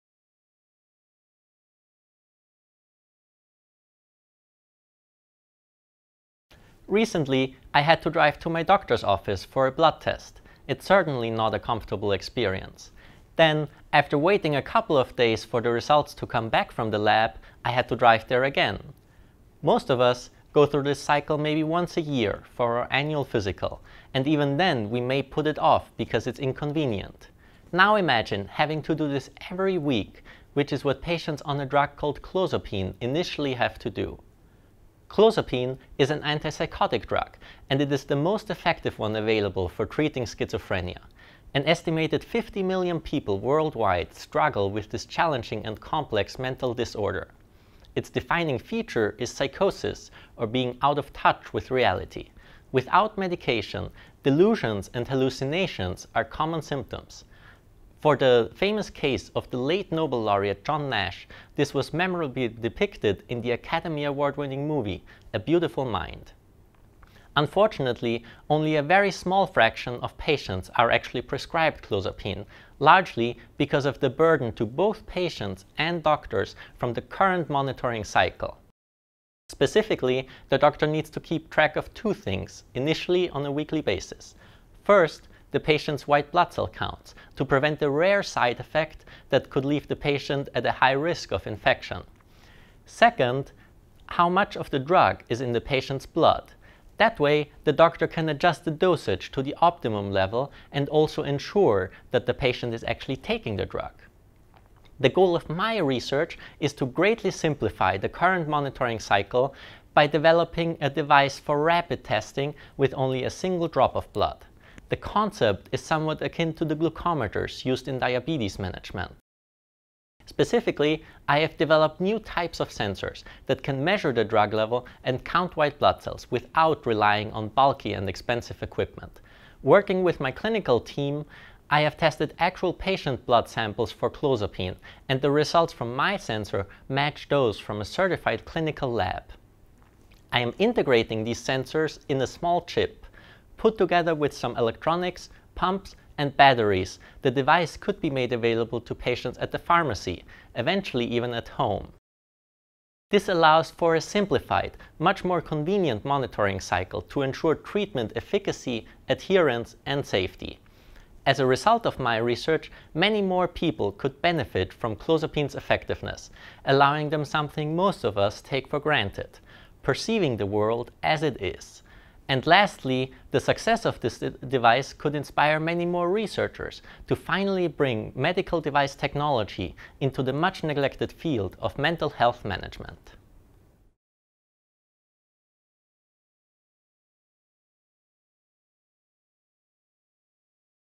Final 3MT Thomas E Winkler 2015
Thomas Winkler, the University of Maryland's runner-up in its contest for the Universitas 21 3 Minute Thesis Contest.